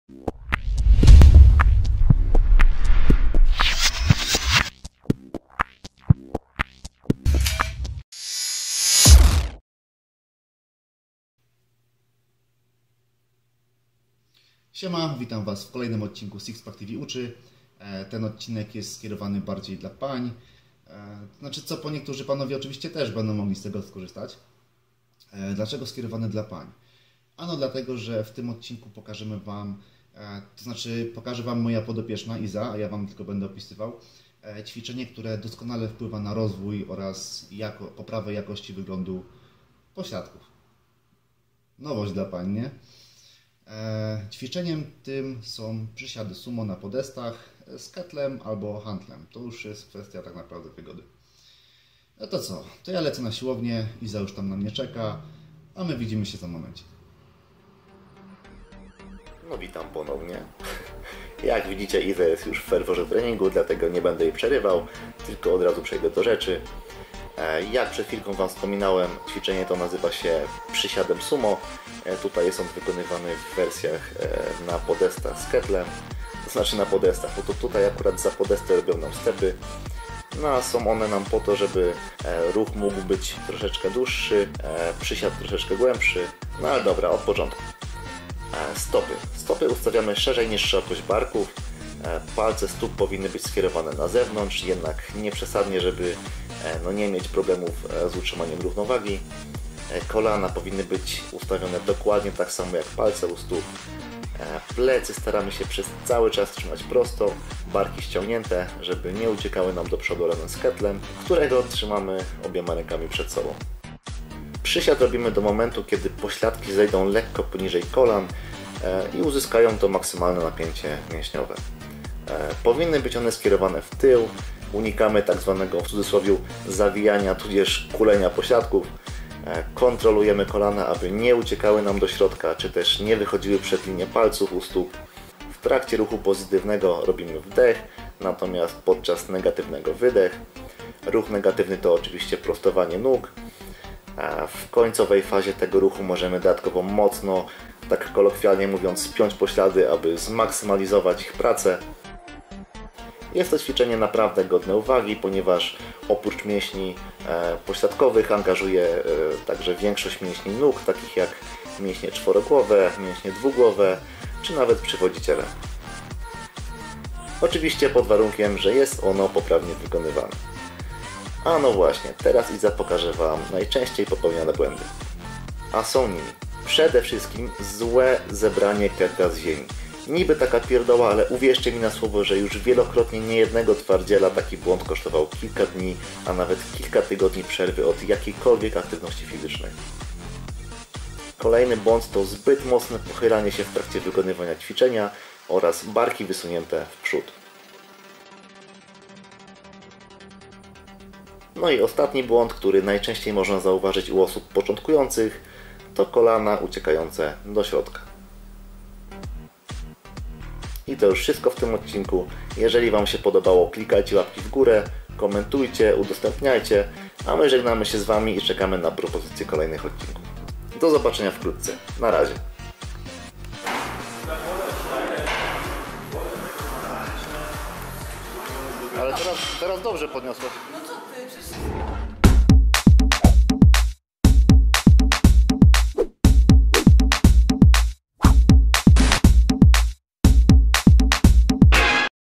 6PAK.tv "ÓCZY" #14 "Przysiady SUMO!"
ODCINEK 14

Pamiętacie jedną z podopiecznych Michała z debiutów podczas imprezy FIT Festival, Izę? Tym razem to właśnie ona wystąpiła gościnnie w naszej serii 6PAK.tv "ÓCZY". Specjalnie dla Was zademonstruje jak prawidłowo wykonać "Sumo squat", czyli tzw. przysiad sumo. Pokaże także czego należy unikać i na co zwrócić szczególną uwagę.